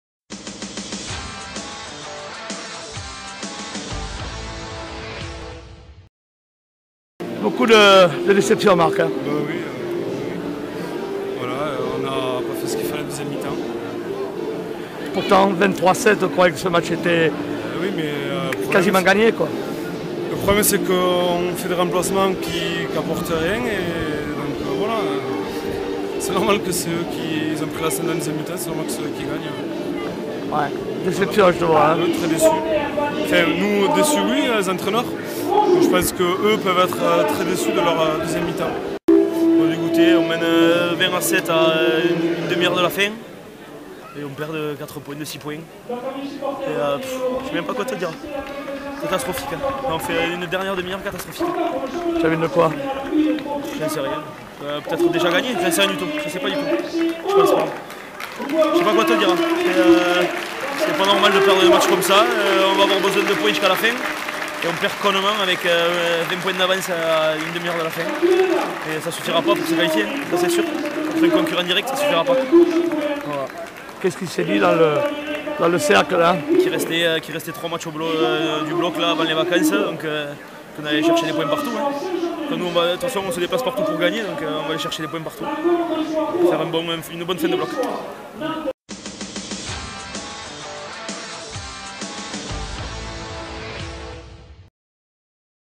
Nationale B. OLORON - BAGNERES
Championnat de France Nationale B - 4ème journée
Oloron 29 - Bagnères 23
Les réactions des Bagnérais après le match
le 16 octobre 2016 - Stade de Saint Pée à Oloron Sainte Marie